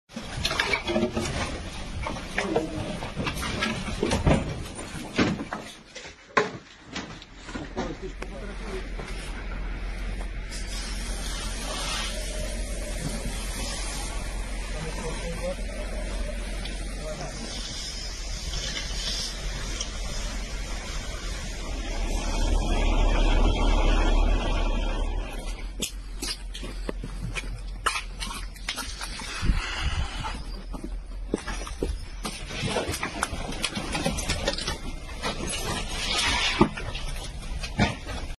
Emergency services look for residents under rubble in Odesa
In Putin's latest airstrike on civilians, an apartment building in Odesa is blown up, killing at least 18 Ukrainians, including two children. In the middle of the night in Odesa, a Russian AS-4 anti-ship missile—the same type that was used to destroy his Kremenchuk mall—was fired at a nine-story apartment building. At least 18 individuals, including two children, are slain.